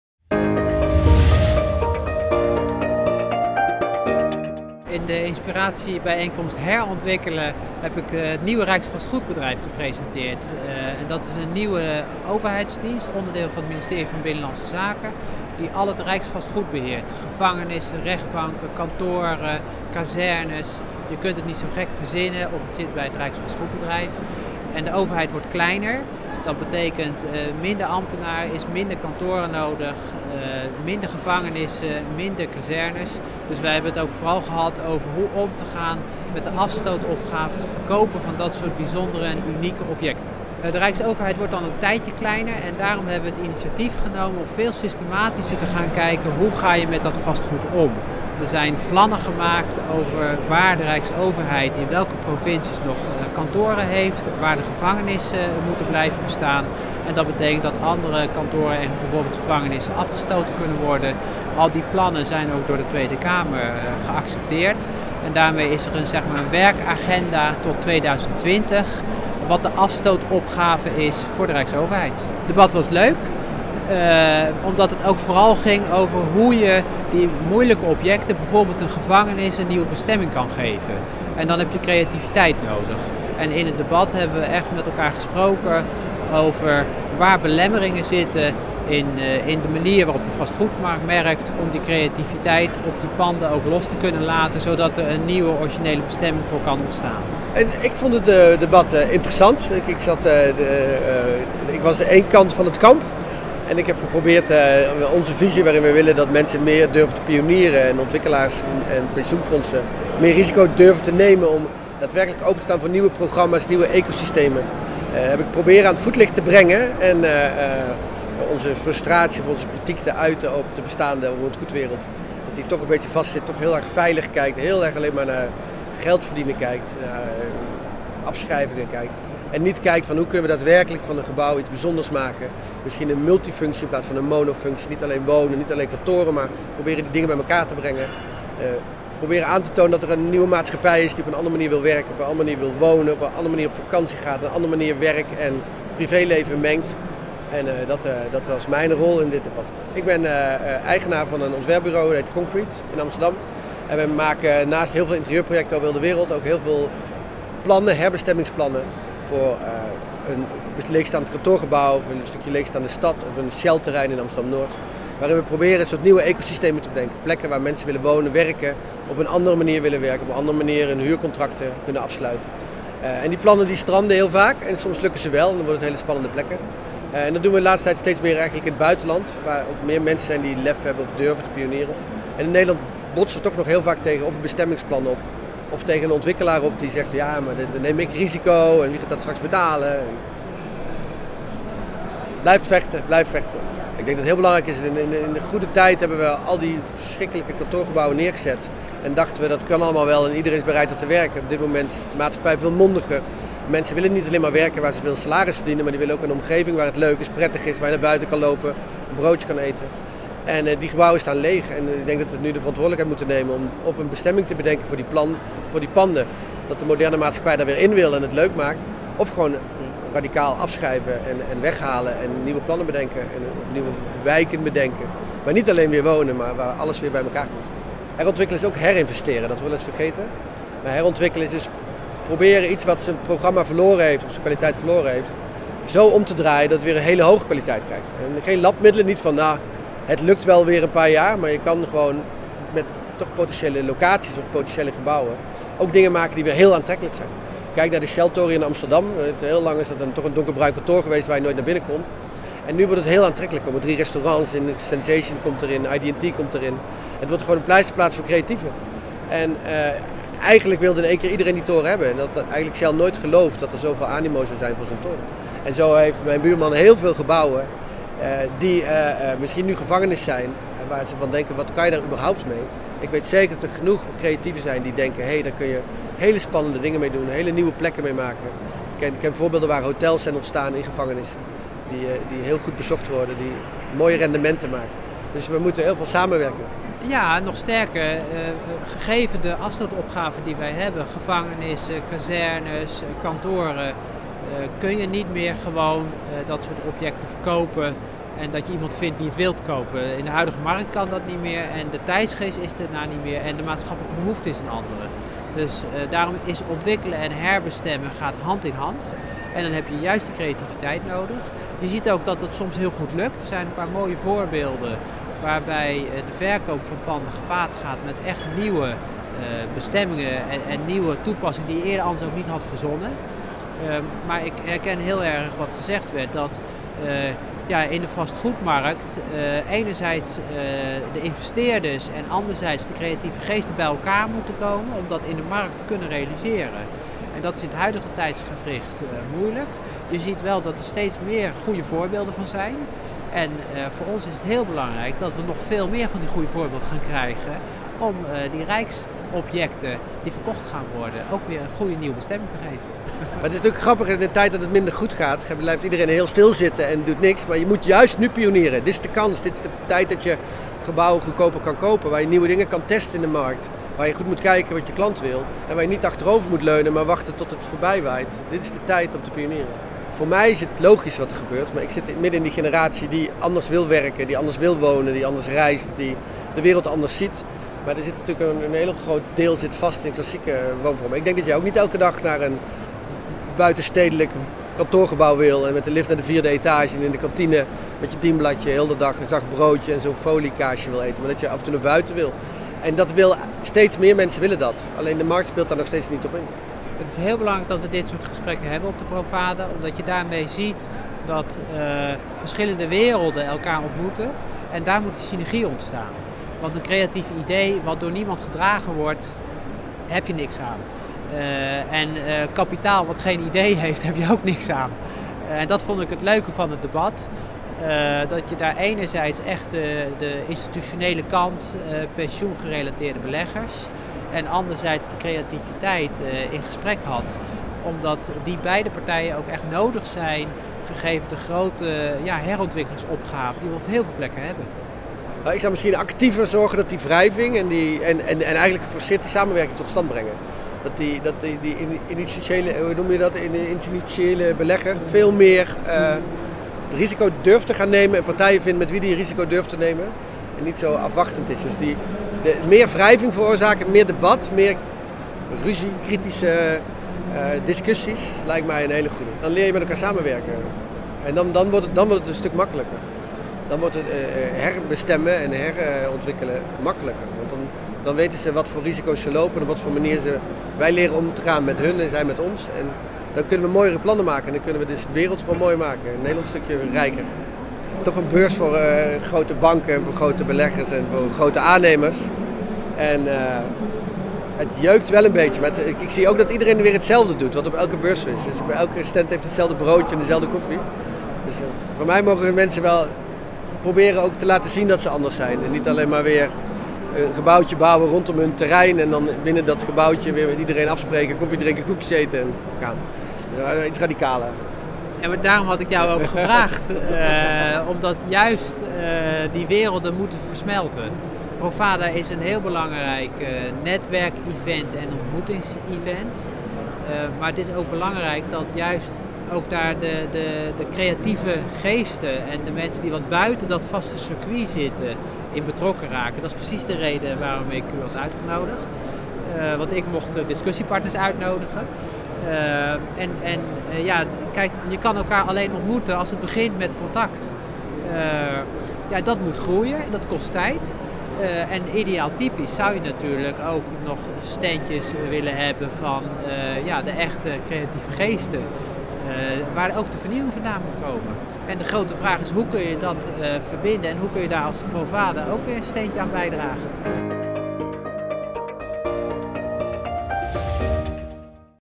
Dinsdag 3 juni Forum Werken
Jaap Uijlenbroek (Rijksvastgoedbedrijf) en Rob Wagemans (Concrete) zijn in gesprek naar aanleiding van de Inspiratiebijeenkomst Herontwikkelen op dinsdag 3 juni in het Forum Werken. Hoe gaat Rijksvastgoedbedrijf om met de grootste vastgoedportefeuille van Nederland en welke creatieve ideeën heeft een partij als Concrete?